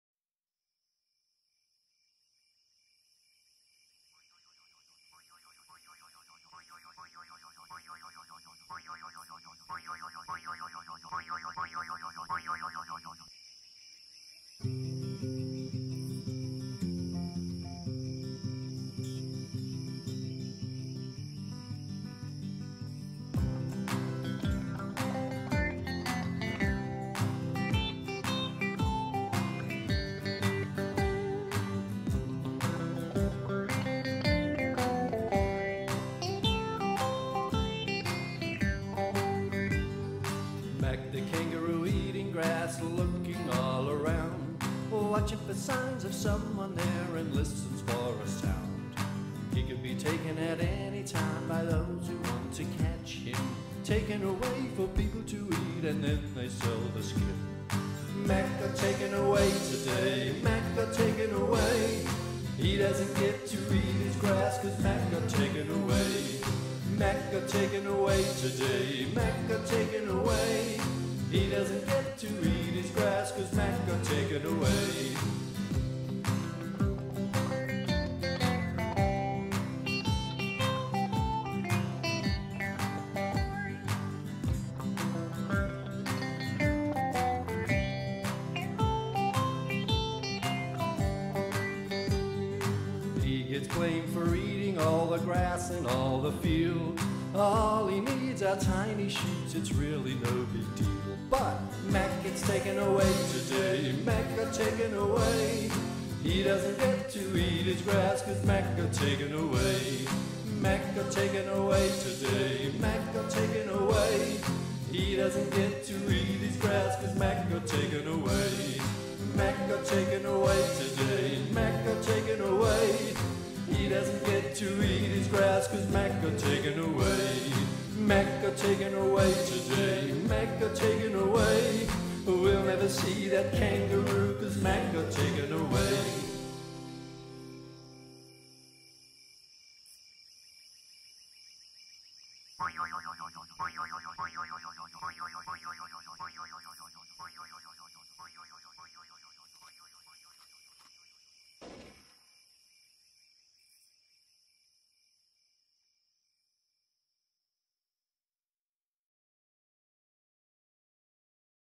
Mac the Kangaroo
Mac the Kangaroo follows the story of one young kangaroo called Mac. He begins his day eating grass with his family and friends. His day ends when he is captured and taken from his family for ever.
"He doesn't get to eat his grass, cause Mac got taken away"